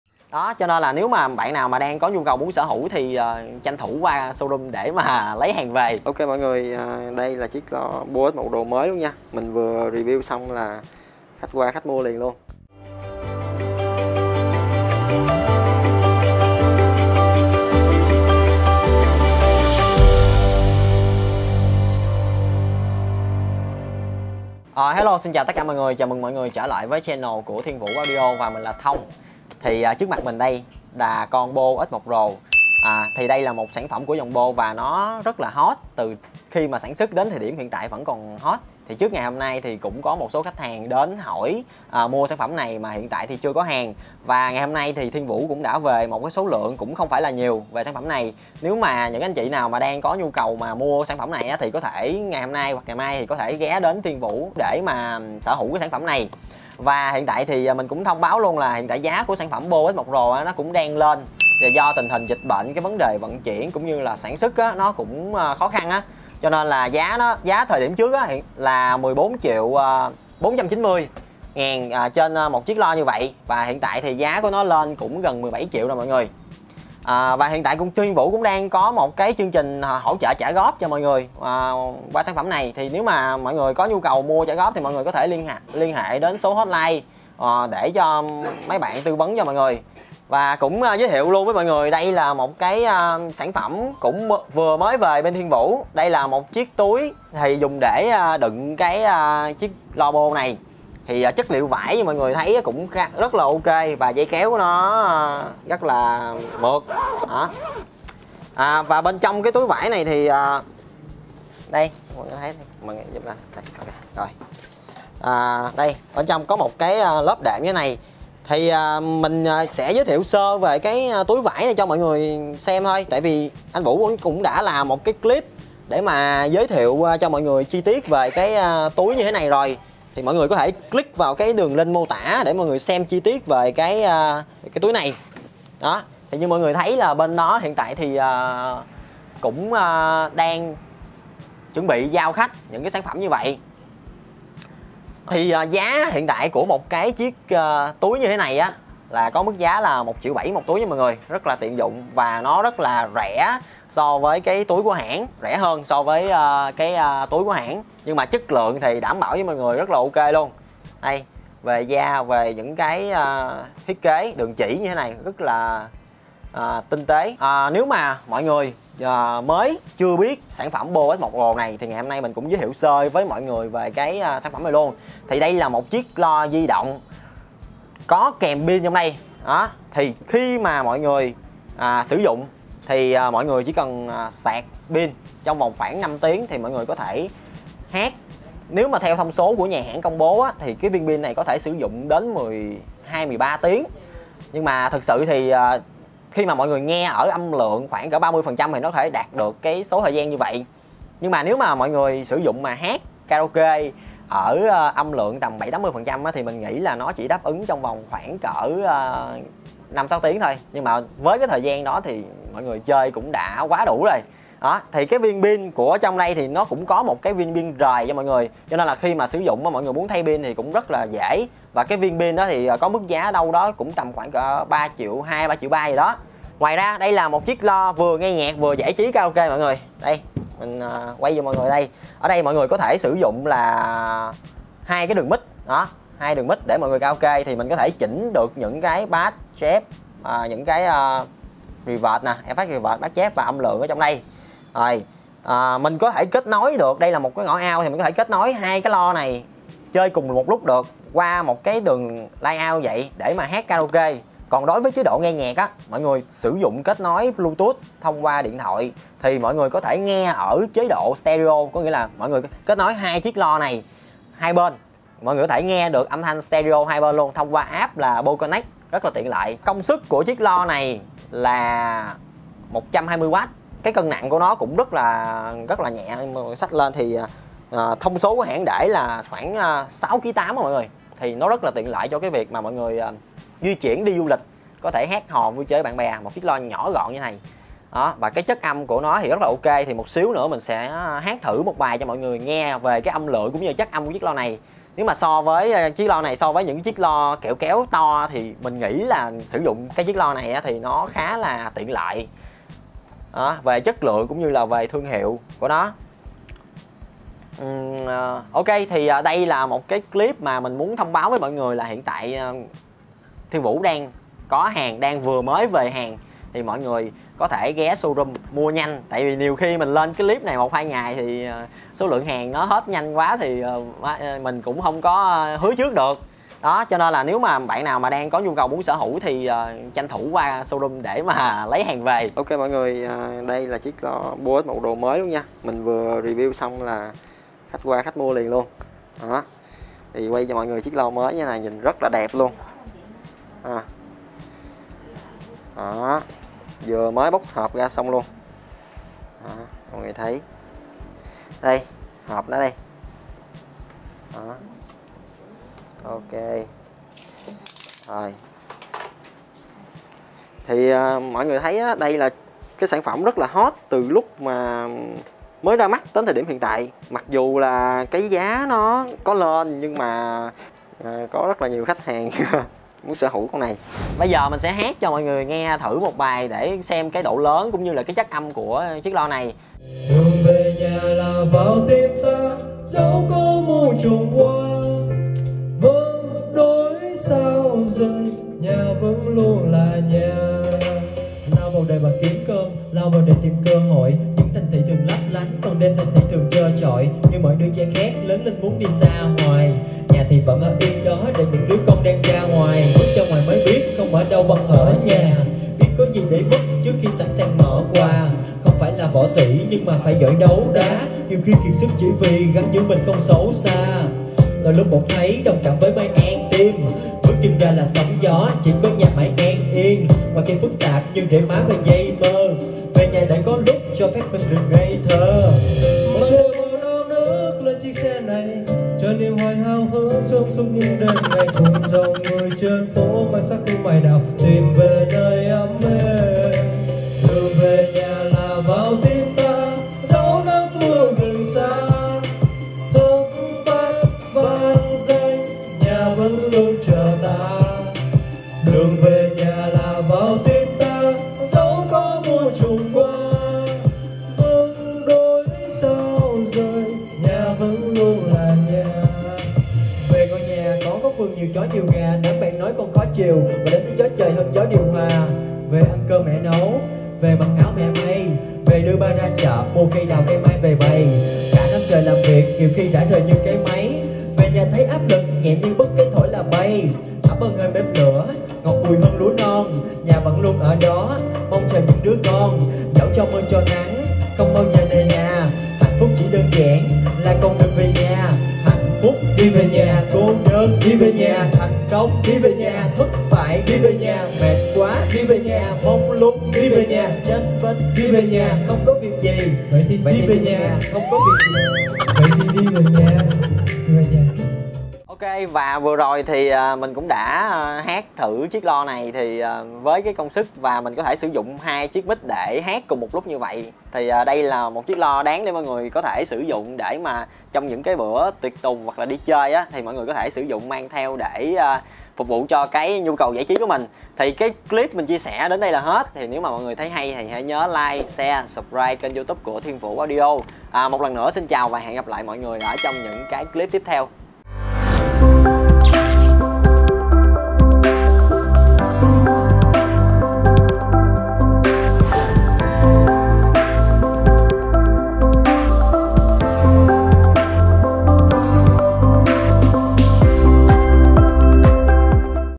Balo Túi Đựng Cao Cấp 2022: Loa Bose S1 Pro đã về hàng !!! Review đánh giá lại chiếc Bose S1 Pro
Balo Túi Cao Cấp: Loa Bose S1 Pro đã về hàng !!! Review đánh giá lại chiếc Bose S1 Pro sau 3 năm